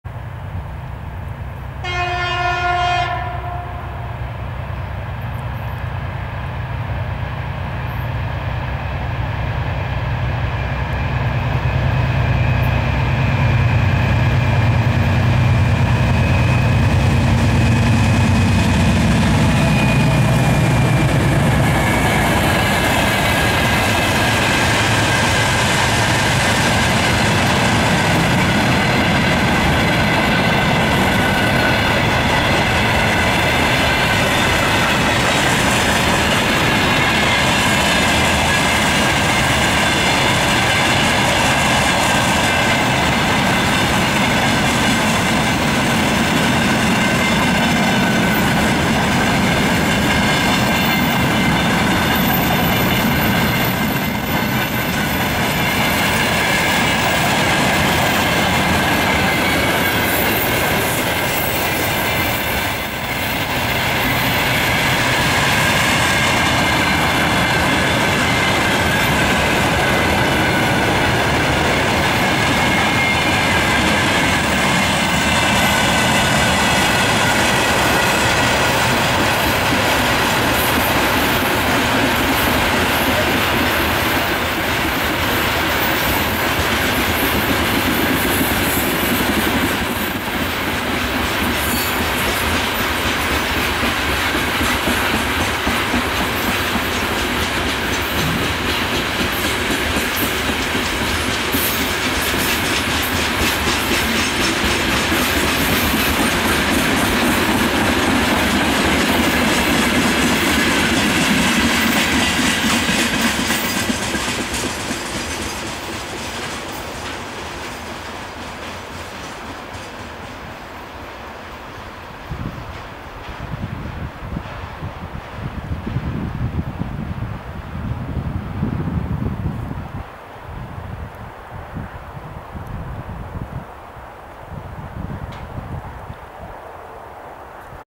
Скрещение тепловозов 2ТЭ10У-0215 и 2M62UM-0094 в о.п.Саулкалне/p.p.Saulkalne
Тепловозы 2ТЭ10У-0215 и 2М62UM-0094 с грузовыми поездами проследуют о.п.Саулкалне/p.p.Saulkalne(участок Саласпилс-Огре)